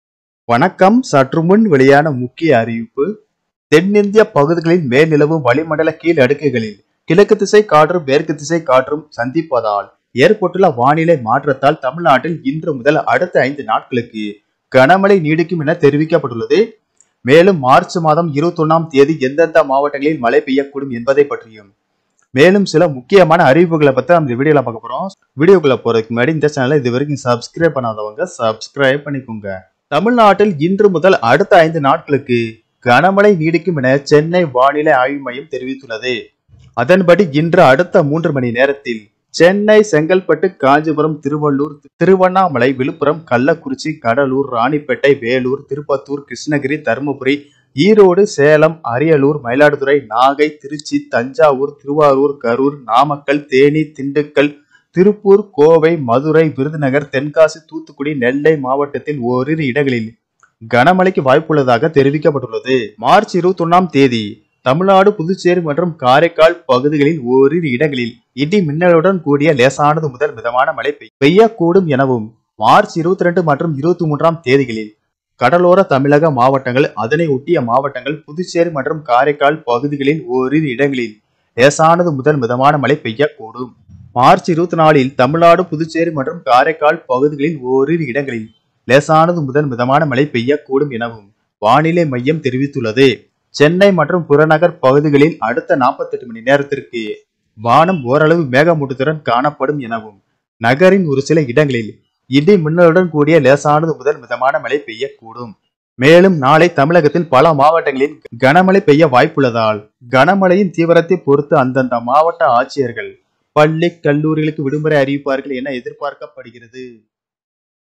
21.03.2023 நாளை புயல் கனமழை எச்சரிக்கை புதிய ஆபத்து விடுமுறை! | Rain | Tamilnadu school leave news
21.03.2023 நாளை புயல் கனமழை எச்சரிக்கை புதிய ஆபத்து விடுமுறை! | Rain | Tamilnadu school leave news

புதிய புயல் கன மழை எச்சரிக்கை பல மாவட்டங்கள் ஆபத்து! பள்ளி கல்லூரி விடுமுறை |  Tamilnadu school leave news | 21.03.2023